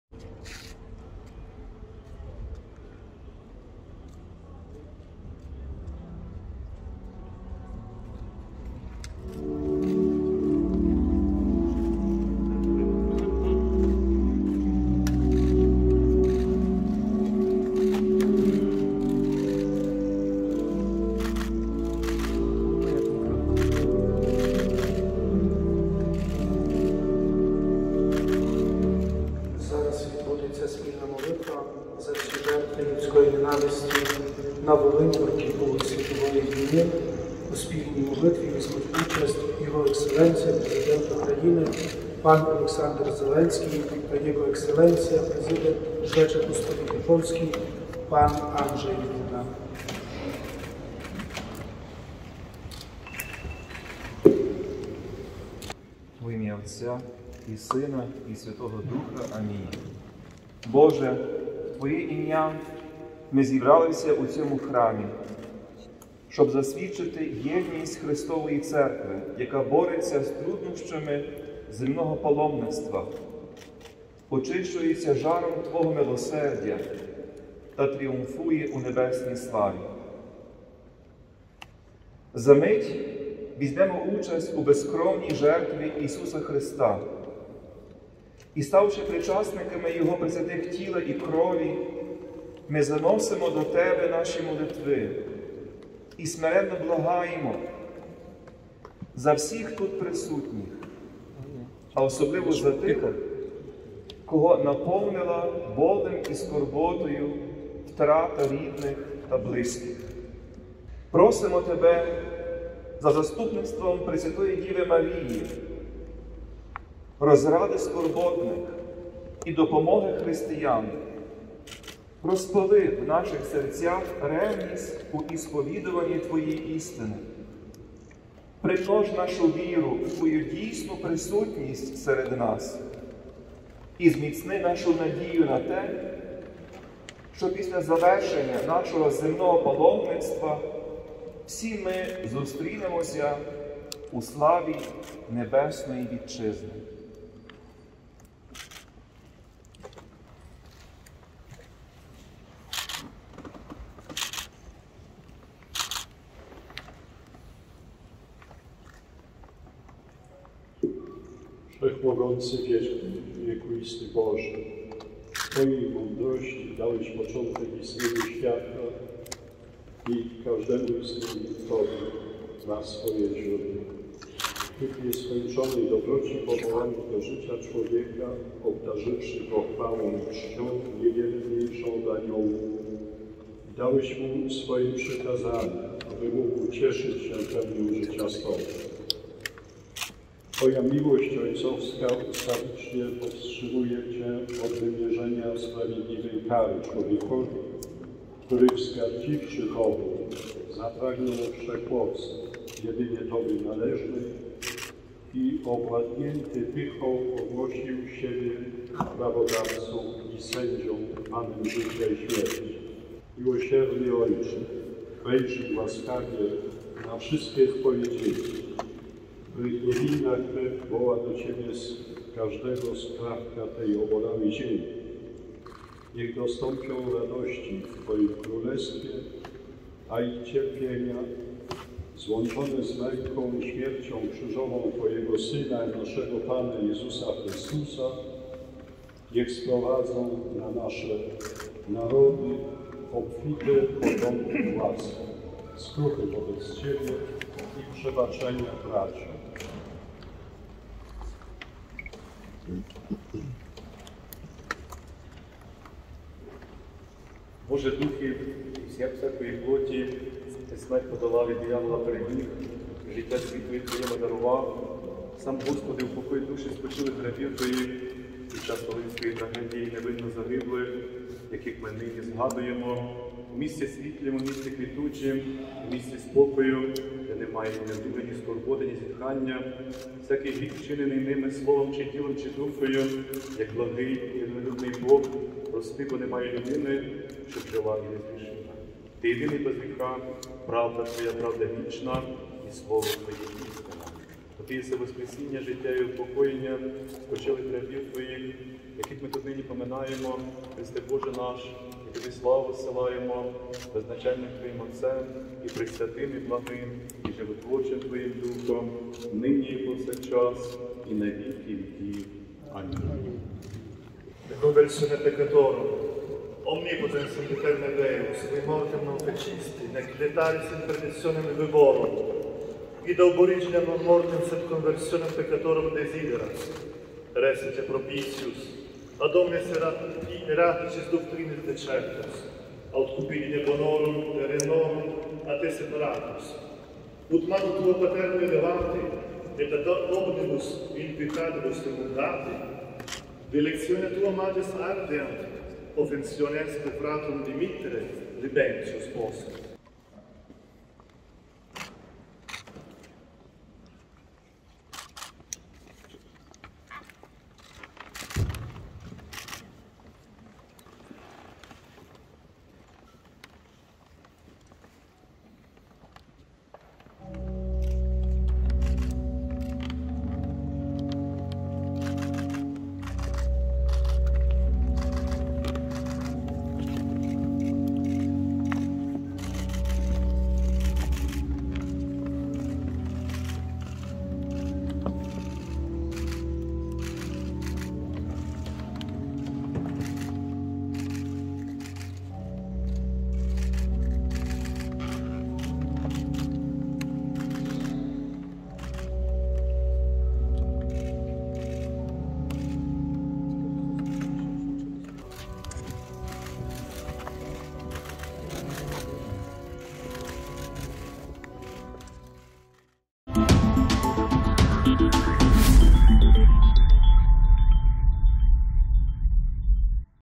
Вшанування пам'яті жертв Волинської трагедії у Луцьку за участю Президентів України та Польщі
Вшанування пам'яті жертв насильства на Волині під час ІІ Світової війни відбулося 9 липня в Луцьку. За участю Президентів України та Польщі, Володимира Зеленського і Анджея Дуди, Глави УГКЦ Блаженнішого Святослава, Предстоятеля ПЦУ Митрополита Епіфанія, очільника єпископату Польщі Станіслава Гондецького, Папського нунція в Україні Вісвальдаса Кульбокаса, представників католицького духовенства України обох обрядів.

Відео Живе телебачення.

Поширюйте відео!

Україна молиться!